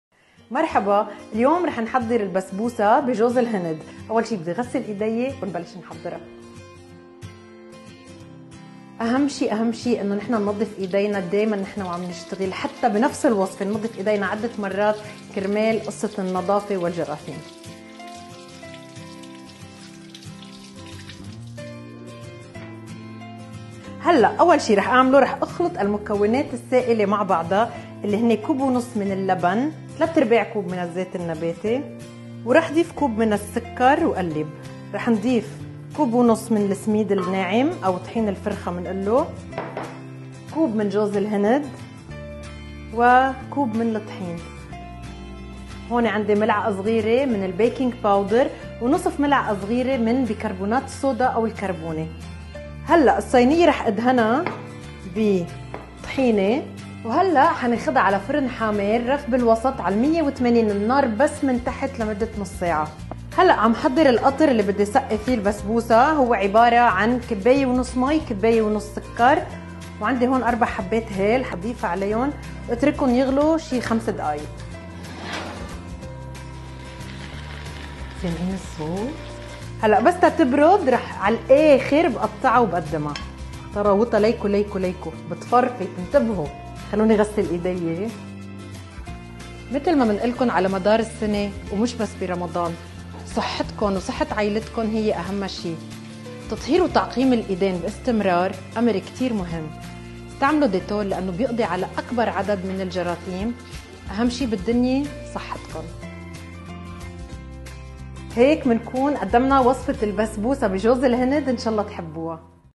الشاف ليلى فتح الله🤩🥮🥥بسبوسة جوز الهند من ام اللول
المكونات:
- 1 و 1/2 كوب سميد ناعم ( طحين فرخة)
- 1 كوب جوز الهند مبروش
- 1 كوب طحين
- 1 كوب سكر
 - 3/4 كوب زيت نباتي
 1 او 1/2 كوب لبن زبادي
- 1 ملعقة صغيرة بايكنغ باودر
- 1/2 ملعقة صغيرة بايكنغ صودا